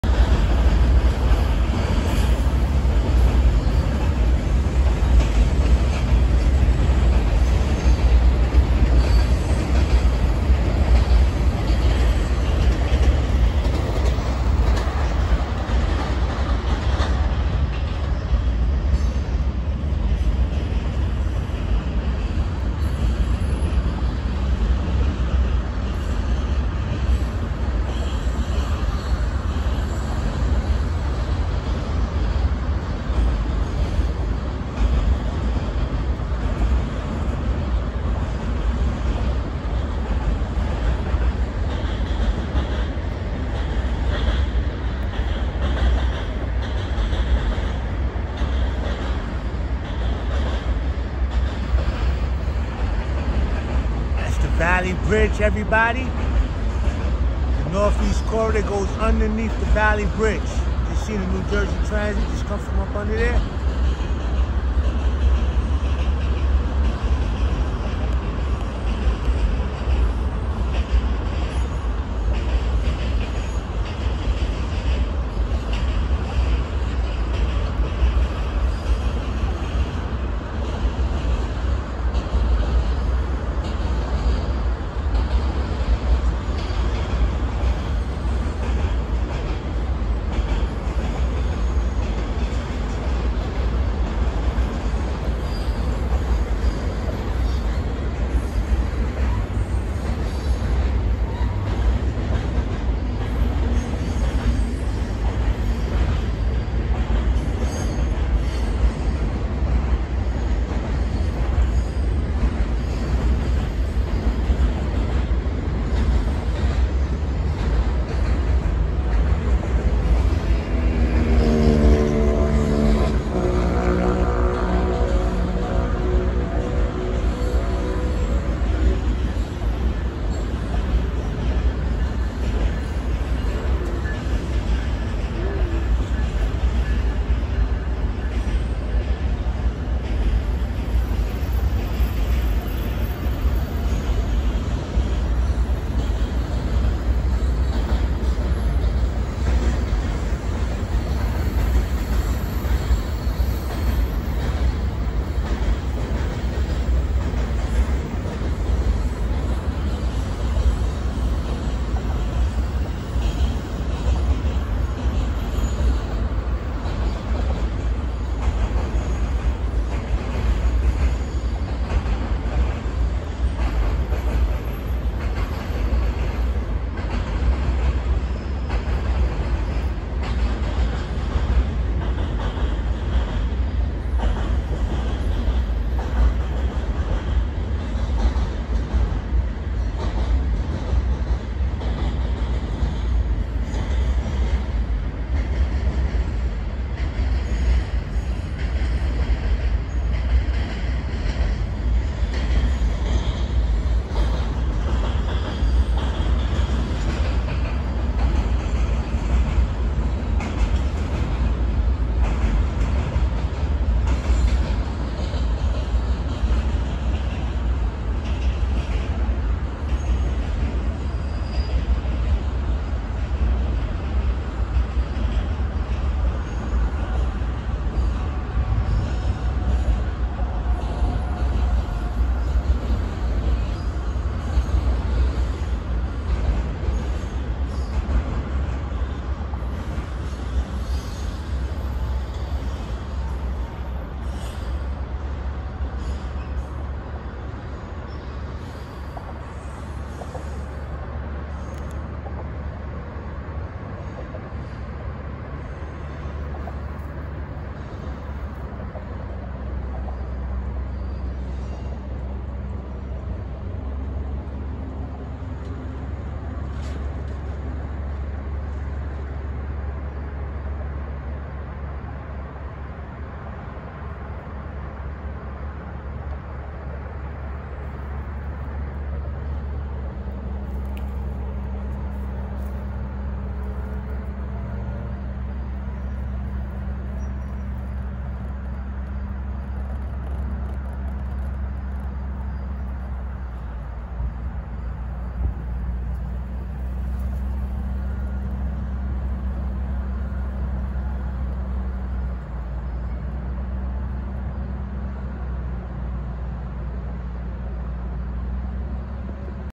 Valley Bridge Action.NJ Transit coming underneath Valley Bridge with CSX on Top
CSX coming across Valley Bridge.NJ Transit on NEC